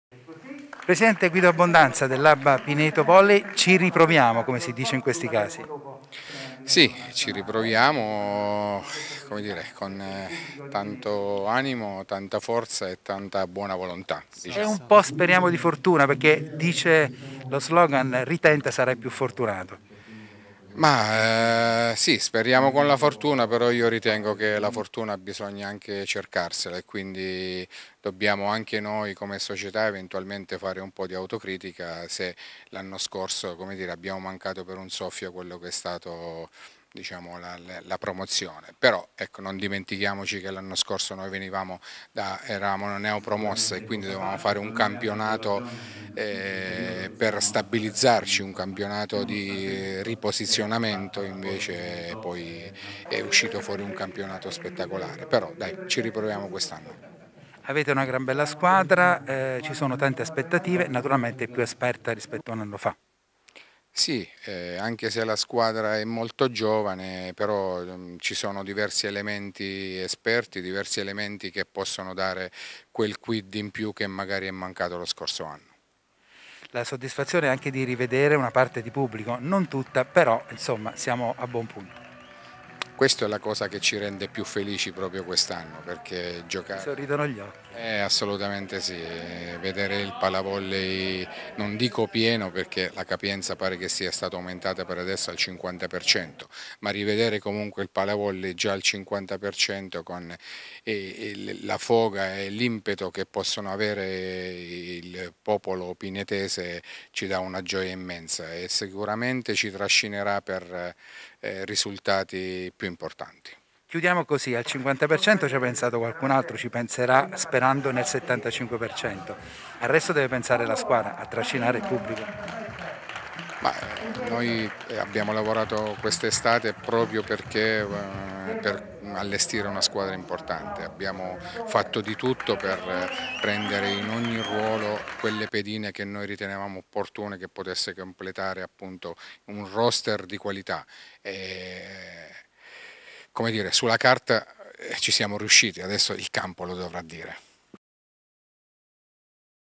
Volley. Presentata la nuova ABBA Pineto
Rinnovamento e ancora maggiori ambizioni per l'ABBA Pineto Volley presentata ieri sera. La squadra del presidente Guido Abbondanza guarda con fiducia al prossimo campionato maschile di A3 sperando di fare meglio della scorsa stagione